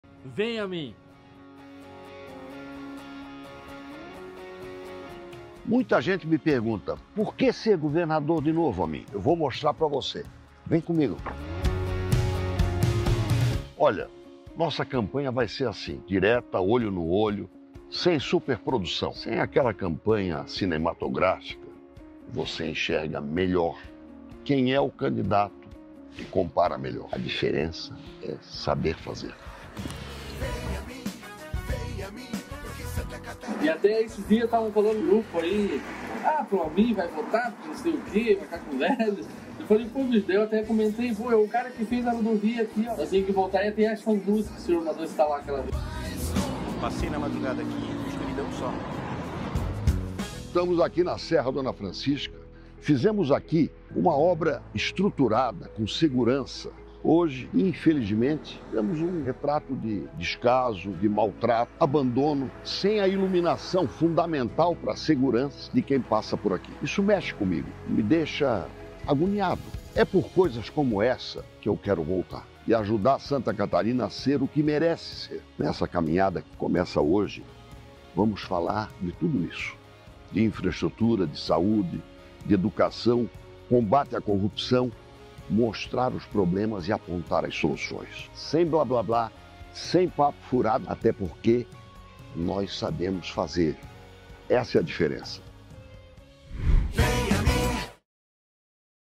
Serra Dona Francisca - Por que ser governador de novo, Amin?
É isso que eu respondo nesse vídeo.

Venham conosco e compartilhem a nossa campanha. A diferença é saber fazer!